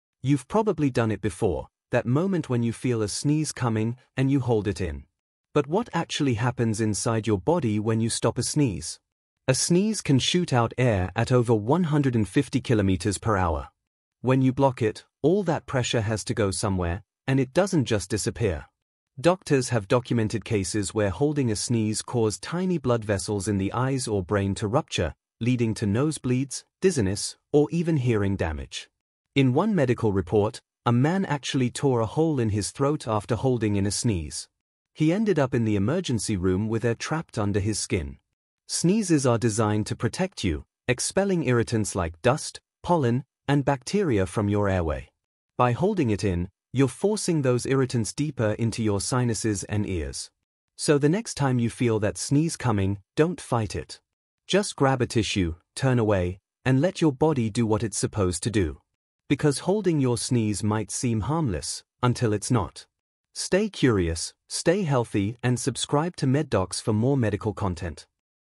Doctors Warn: Don’t Hold Your Sneeze
What actually happens inside your body when you hold in a sneeze?

This short breaks down how much pressure builds up in your sinuses, why holding your sneeze can damage blood vessels, and even the rare cases where it can injure your throat.

MedDocsAI brings simple, fast, science-based medical education using AI visuals and clear explanations.

Stay curious. Stay healthy.